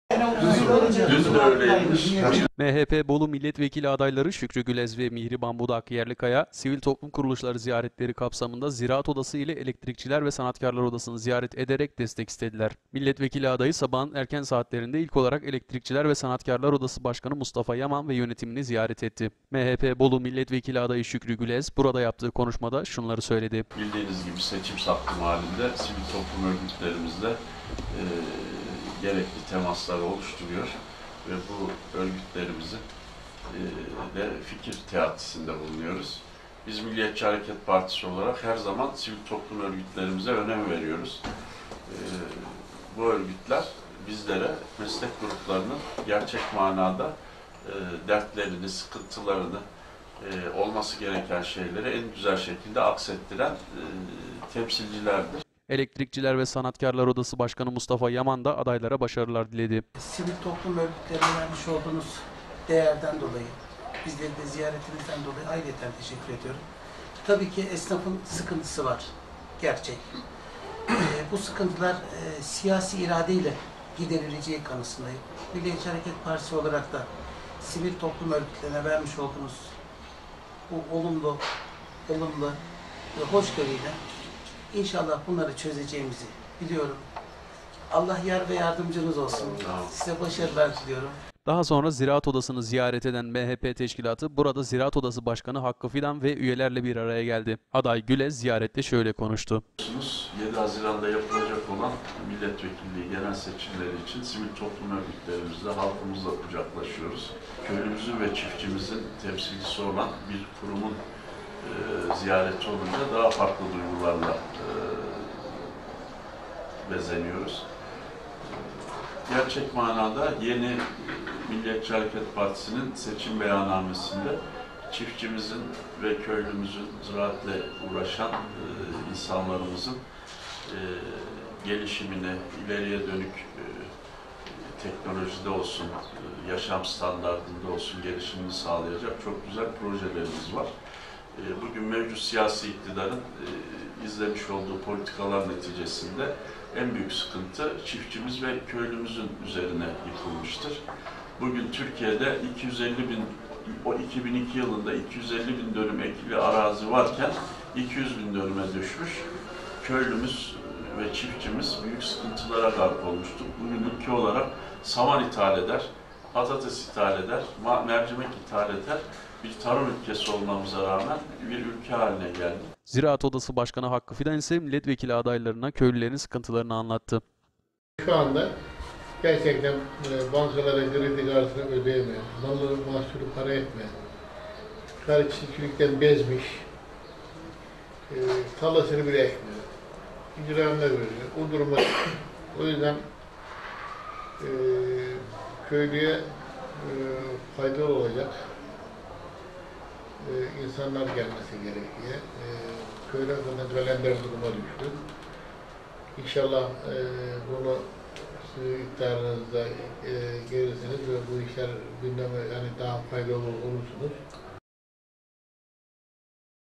MHPLİ ADAYLARDAN STK’LARA ZİYARET
MHP Bolu Milletvekili adayları Şükrü Gülez ve Mihriban Budak Yerlikaya, sivil toplum kuruluşları ziyareti kapsamında Ziraat Odası ile Elektrikçiler ve Sanatkârlar Odası’nı ziyaret ederek destek istediler.